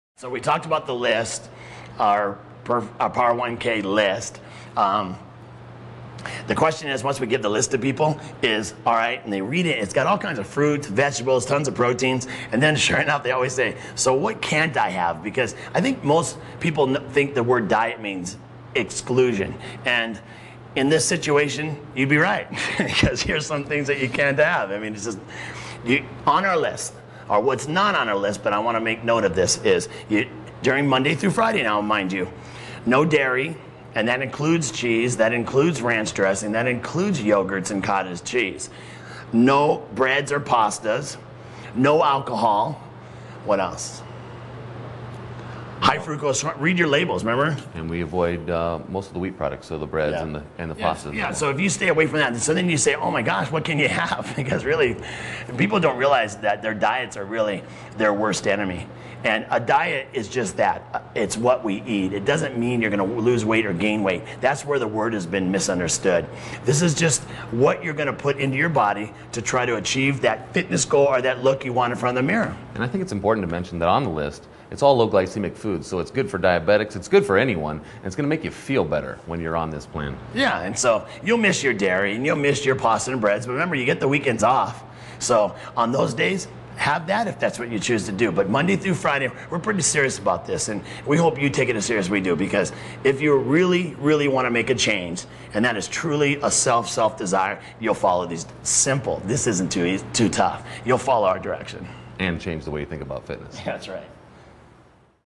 What Can't I Eat?
Bob Mathews & Matt Jarvis of speak about foods that are harmful to your weight loss goal.  Through years of experience, they have combed through the mis-truths and lies and developed an eating plan that is realistic and it works!!!